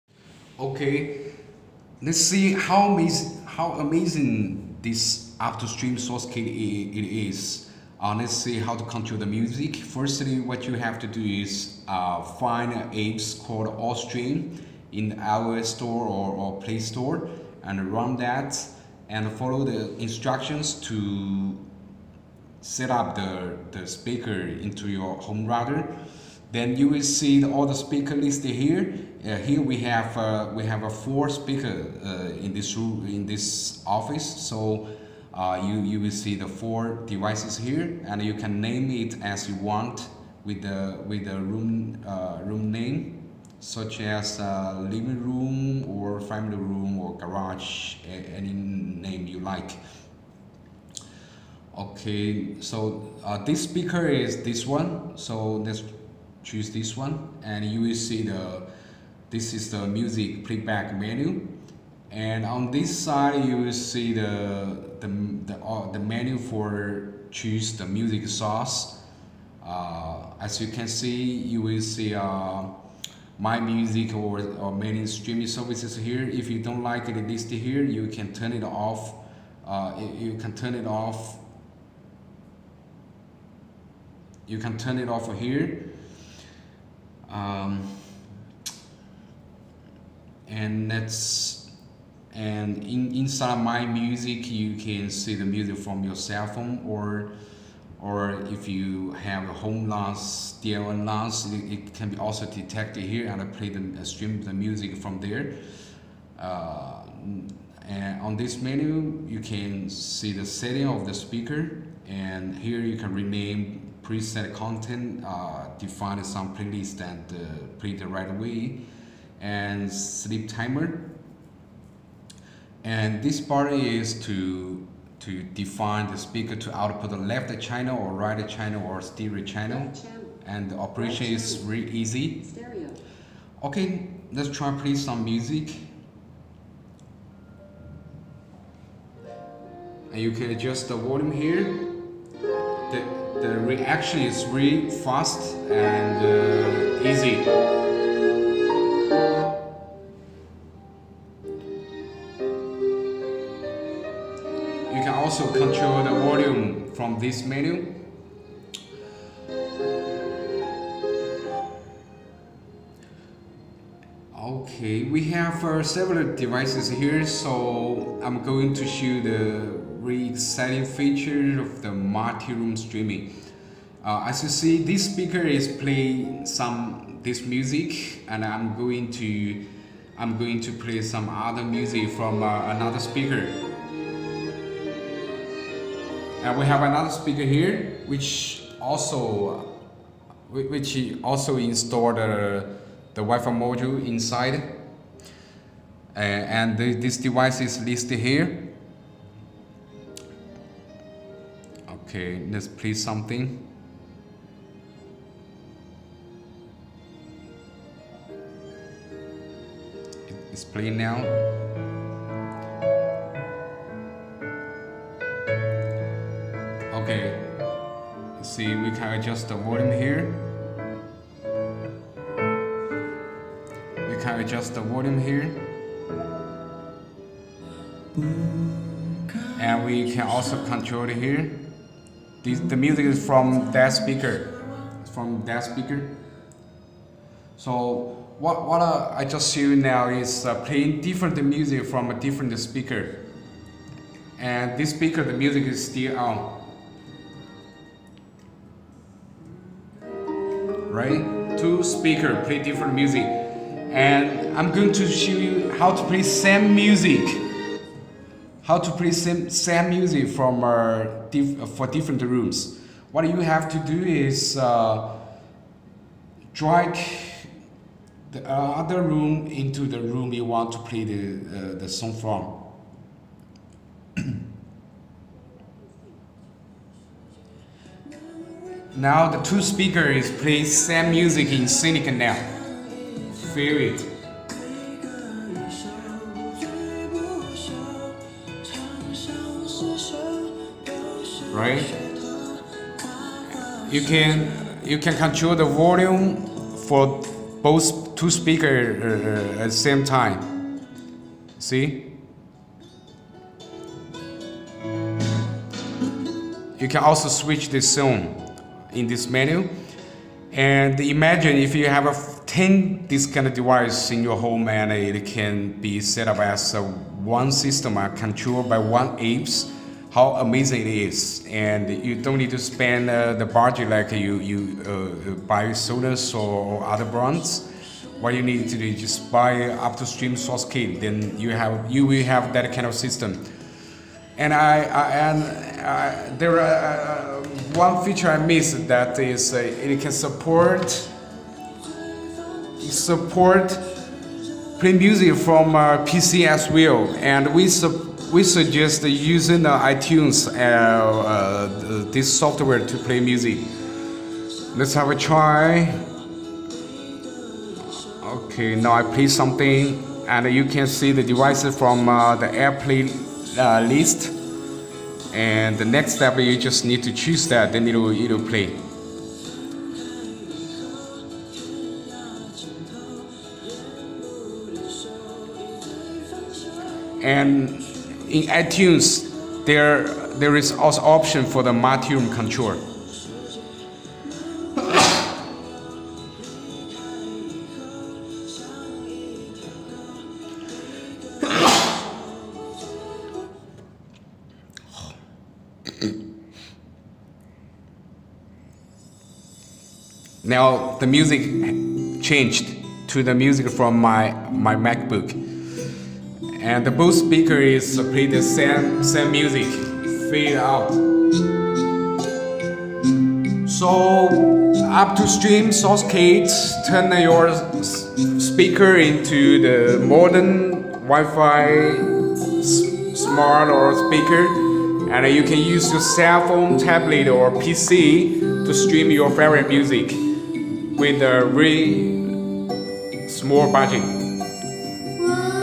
Up2Stream Multiroom WiFi Audio Receiver App Demo
Up2Stream Source Kit Introduction
Up2Stream Source Kit is a tiny 2.4G wifi audio receiver DIY module with 2 pin analog output and 2 pin power input. It’s designed to be installed easily inside existed stereo active speakers by the users themselves ,transforming the speakers into wireless or smart speakers.It will give new life to the speakers , tremendous new features and upgrade the sound quality to a much higher level .

Here are some mean features it offer :
Stream music from cellphone,tablet,PC by wifi with distance limitation .
Stream high quality lossless music files via wifi ,such as Flac,WAV
Music files with 24bit/192KHz supported
Support streaming protocol,dlna,Airplay,spotify connect,Qplay.
Support Multi-Room/Multi-Zone music streaming,ex play same music in sync in all room or different  music in different room or different zone group.
Support stream from different sources ,such as phone memory ,NAS or streaming services .
Free Android&iOS app available with online update for new features .
Support streaming from Spotify,Tidal,iHeartRadio,Tunein,QQMusic,Ximalaya
More features and music services available by future online update
More interesting features ,such as music alarm, pre-set playlist ,stereo pair.
API available for home automation dealers for integration .